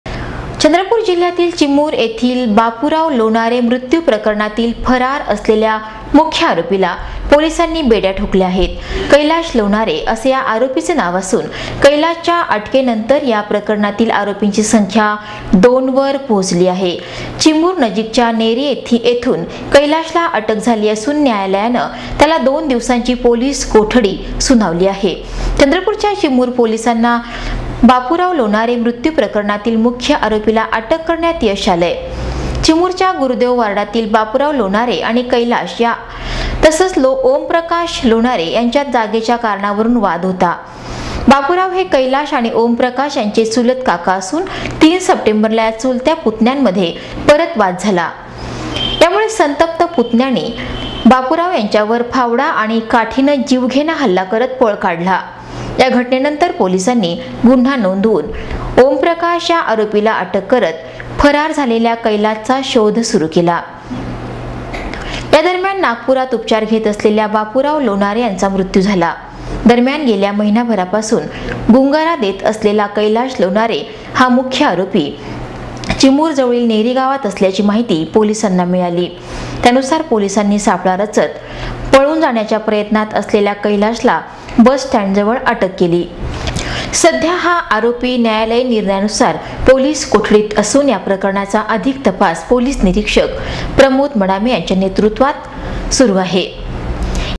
*चिमूर चंद्रपूर- कोणत्या मुख्य आरोपीच्या पोलिसांनी आवळल्या मुसक्या
*चिमूर चंद्रपूर- कोणत्या मुख्य आरोपीच्या पोलिसांनी आवळल्या मुसक्या, किती आहे या खून प्रकरणातील आरोपीची संख्या, बघा हि बातमी सविस्तर